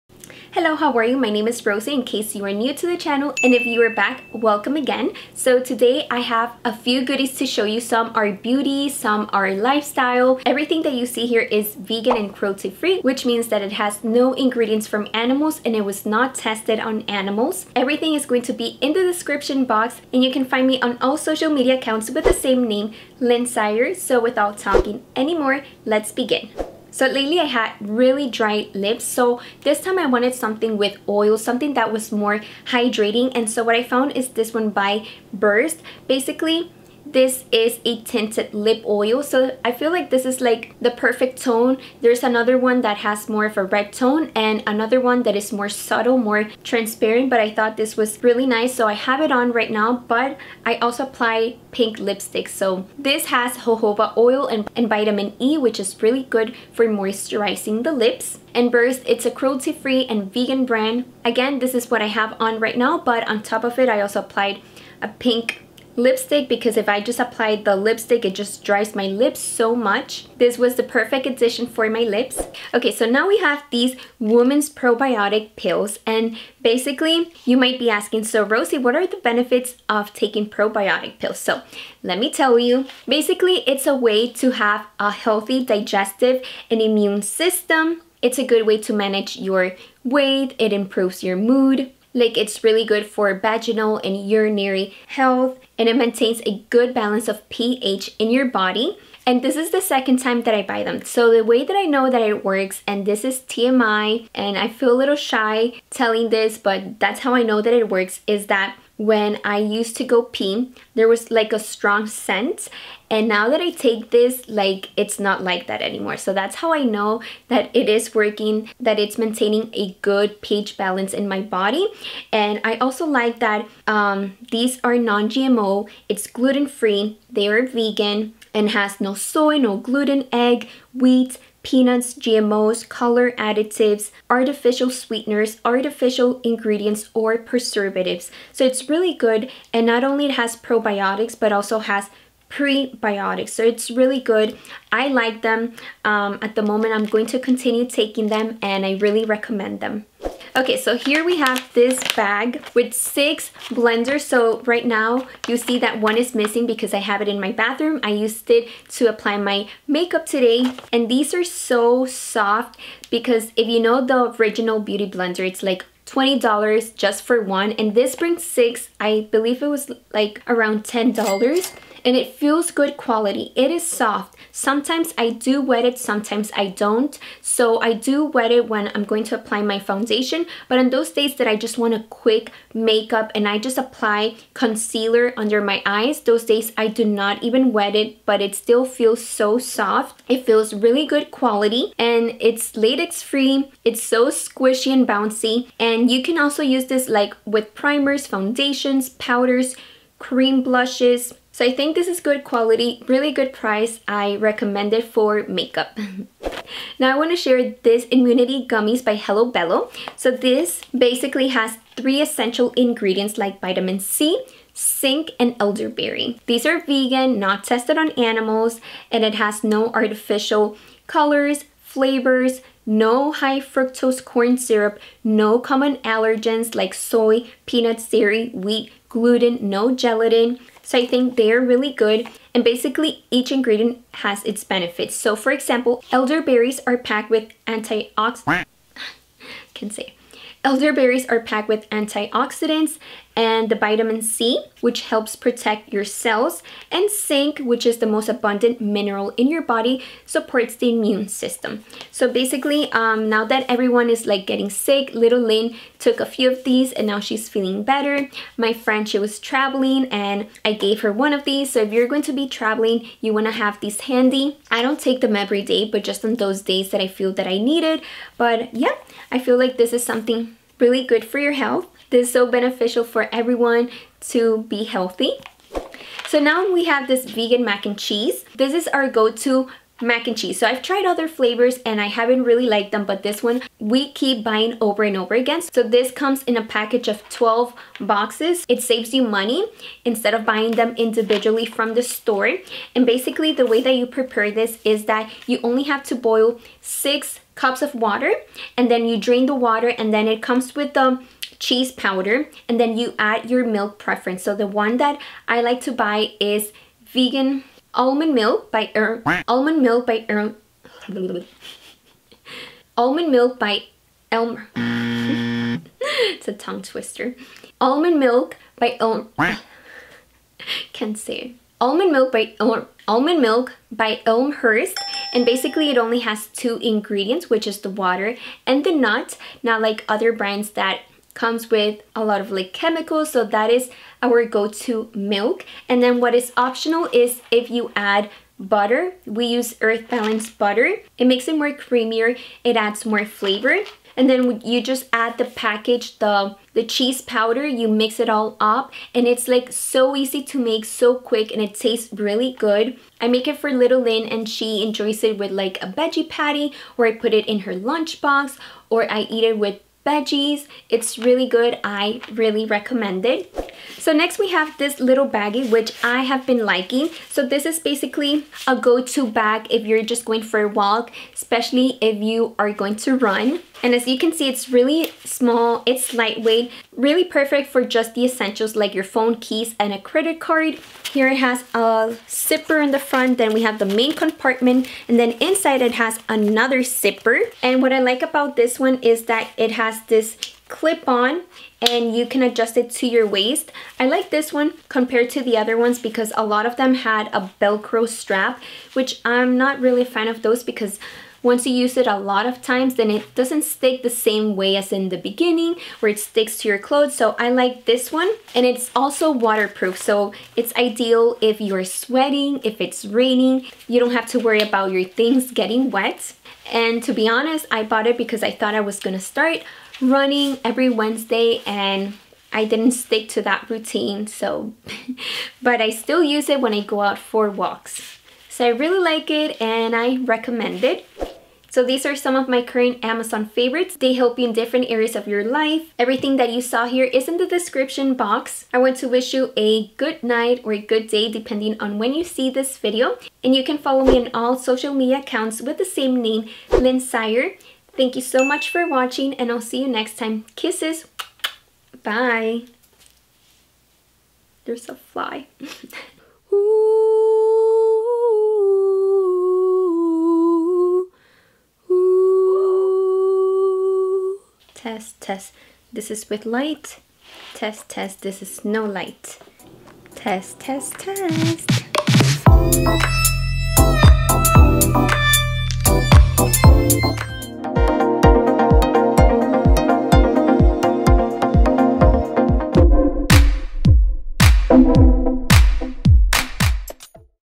Best Amazon Must Haves 2022 Under $35 | LynSire
In this video I share with you 6 Amazon must haves. Some are beauty related and some are lifestyle. Most of these are under $20, with the exception of one under $35 because it comes as a bulk item.

PRODUCTS MENTIONED
Versed Silk Slip Tinted Lip Oil, Blossom
Probiotics for Women with Organic Prebiotics
Makeup Sponge Set
Hello Bello Immunity Vitamins
Annie’s Vegan Mac and Cheddar Flavor
Running Fanny Pack

Ruffle beige top
Gold hoop earrings
Rose Gold Michael Kors watch

Much love,
Rosy (LynSire) ♡